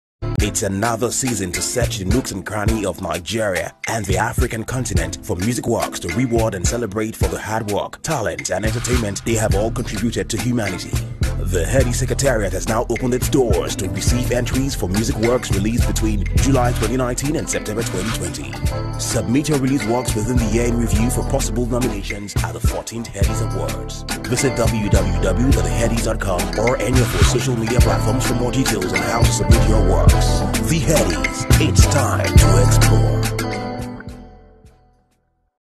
The Headies Awards (14th Edition) | Songs submission
As Nigeria’s most revered music award show and a custodian of Nigeria’s music heritage, The Headies Awards is continuously tasked to identify, celebrate and reward hardworking music creators and contributors to the entertainment industry.

This year will mark the fourteenth edition of the Awards, and we have opened our doors once again for the submission of all music works released between July 2019 and September 2020, the year-in-review.

After years of consideration, we are delighted to announce that we will be including one new nomination categories – “Songwriter of the year”, to bring to the limelight hardworking individuals who have all significantly supported the creation of quality music.